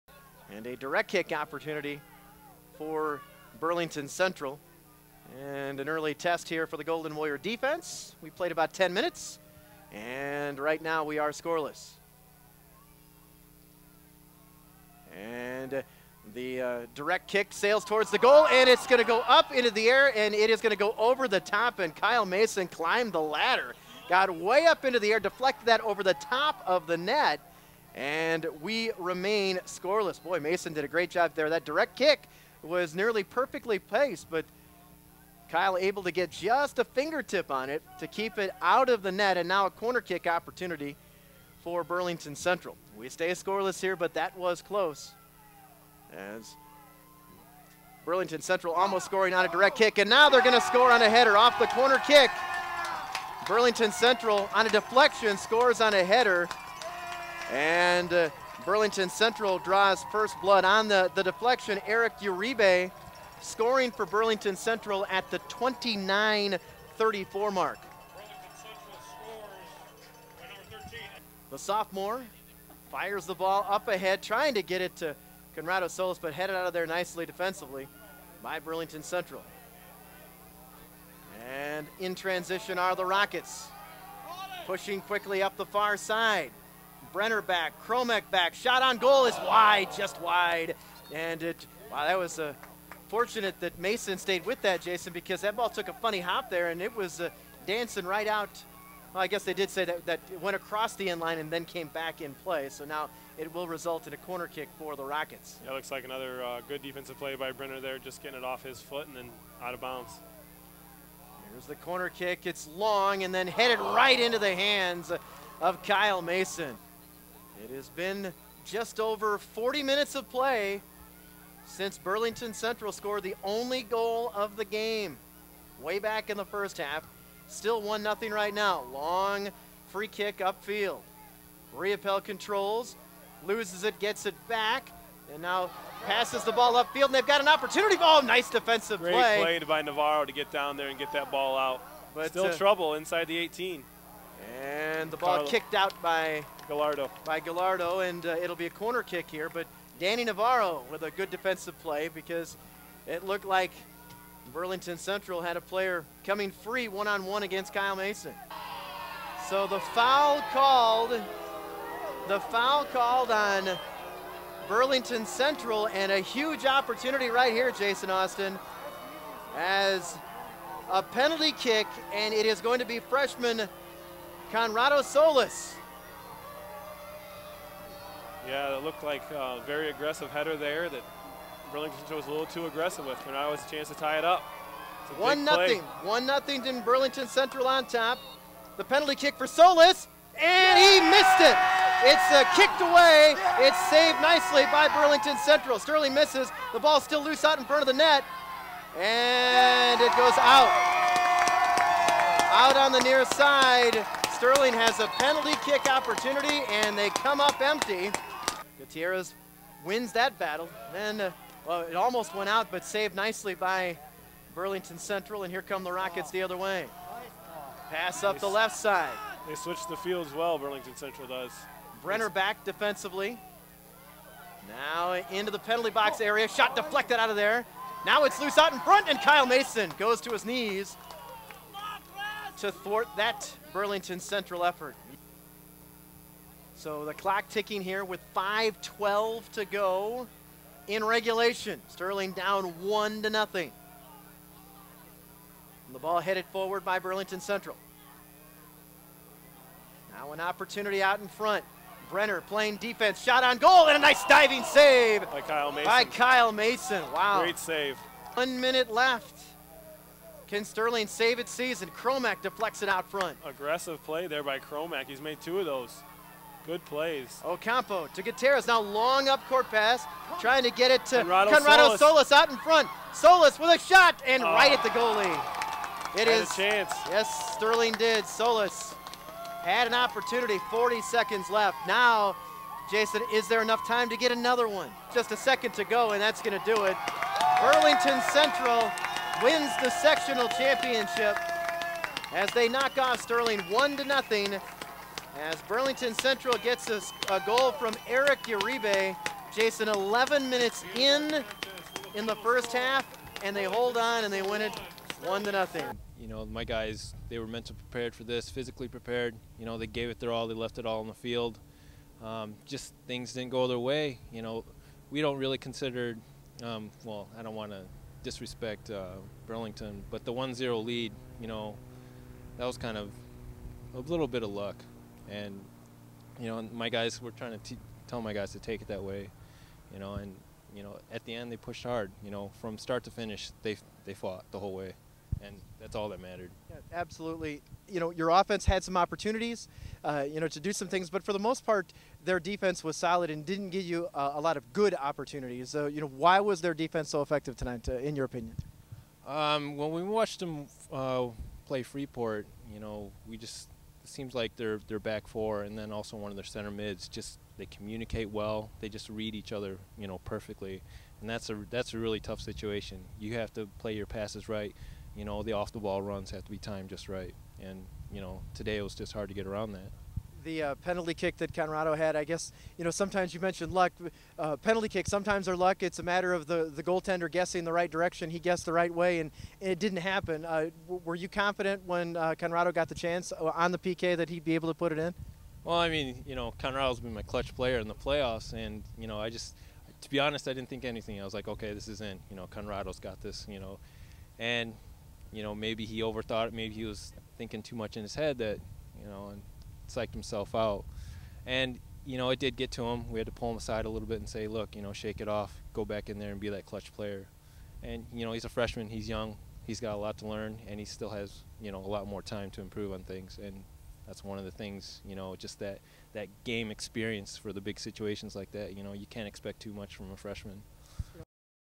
SHS Soccer Vs Burlington Central 10-29-2010
The SHS Boys' Soccer team falls to Burlington Central in the Sectional finals.